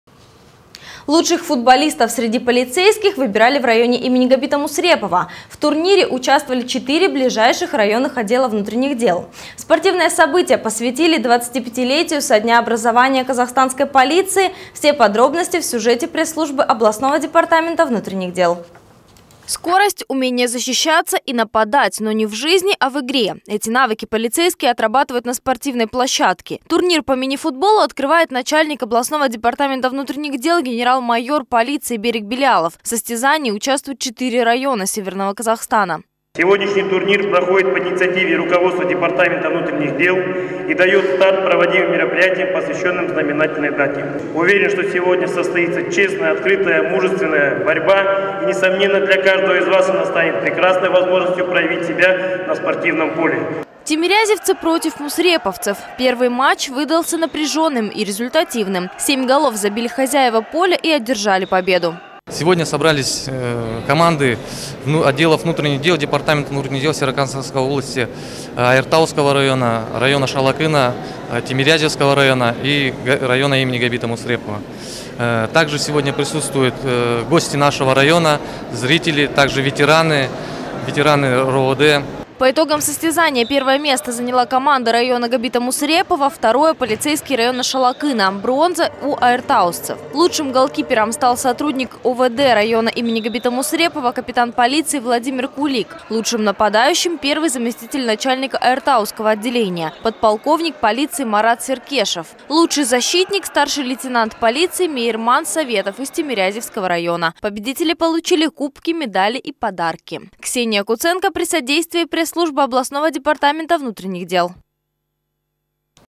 рус ДВД Мини футбол
Лучших футболистов среди полицейских выбрали в районе им. Г.Мусперова. В турнире участвовали четыре ближайших районных отдела внутренних дел. Спортивное событие посвятили 25-летию со дня образования казахстанской полиции. Все подробности - в сюжете пресс-службы областного ДВД.